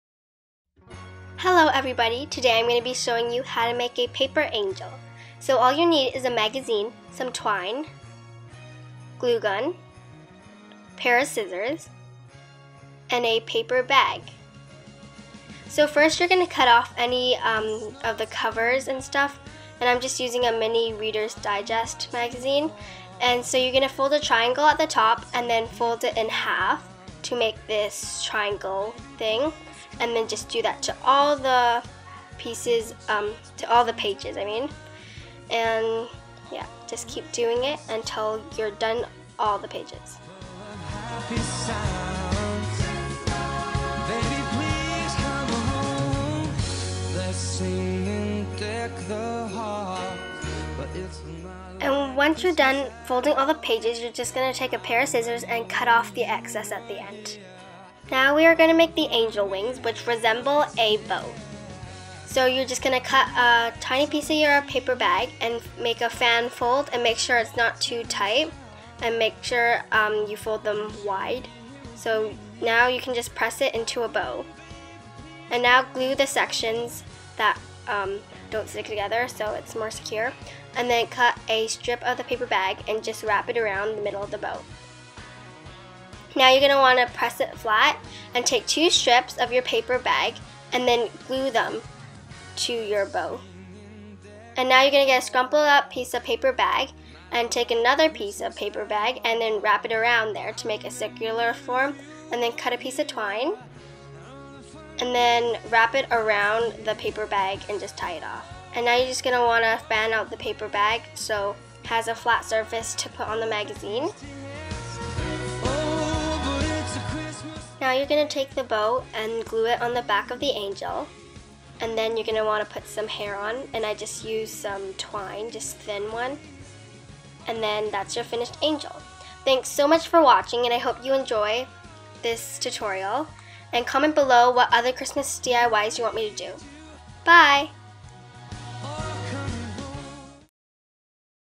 DIY Paper Angel
Hello Everybody!

Sorry if I sound a little bit weird in this video, I'm just getting over a cold. This project is perfect if you have those extra magazines lying around that you don't typically use them. It is a great recycling project and is perfect for any household.
-Alli